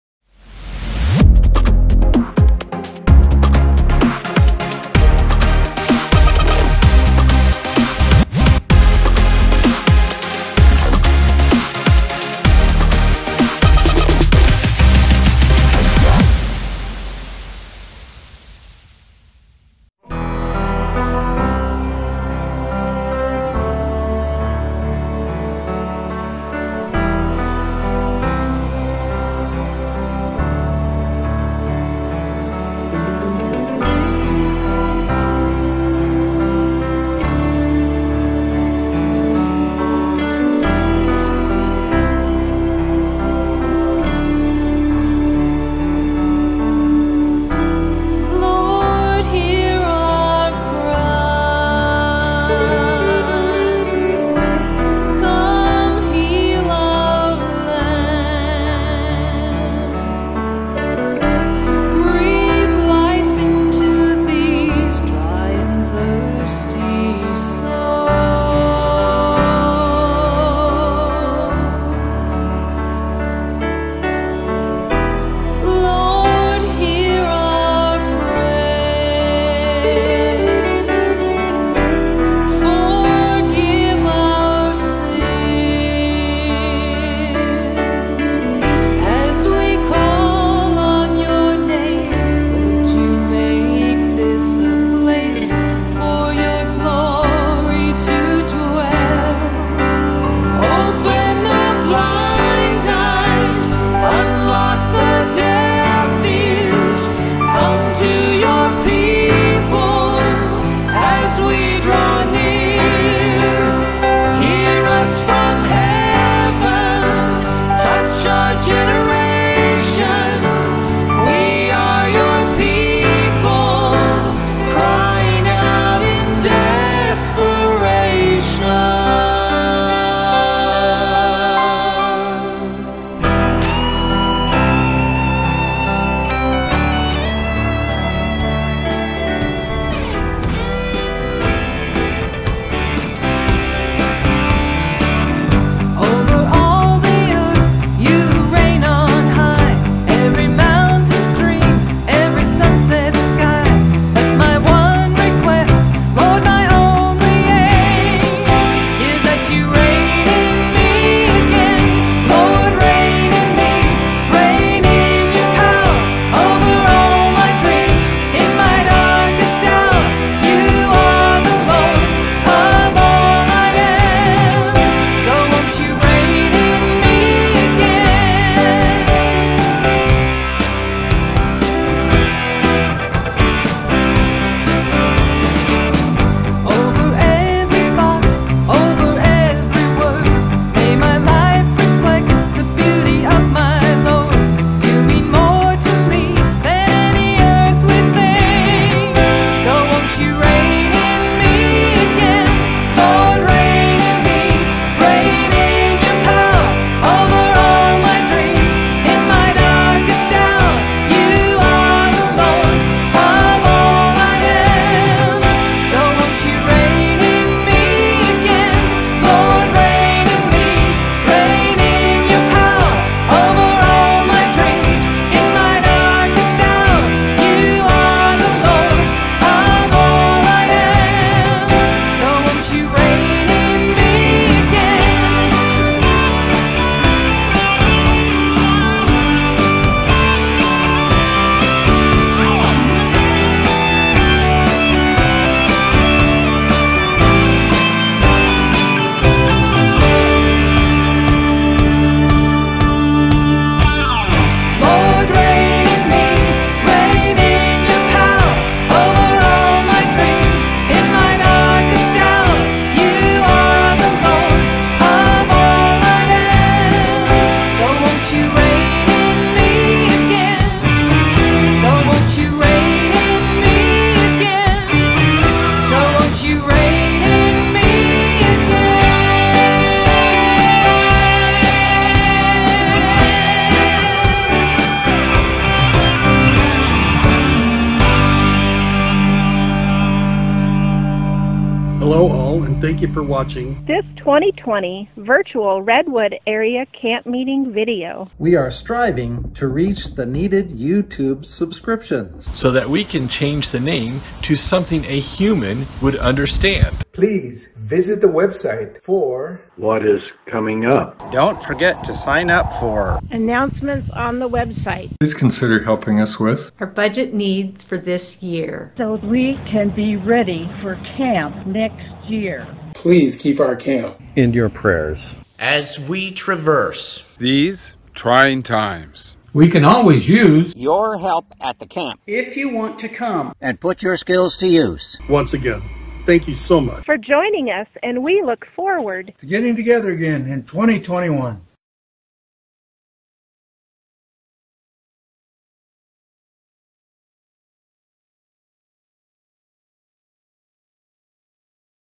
Prayer Walk
Walk around the Redwood Campmeeting grounds through slides while listening to Mosaic music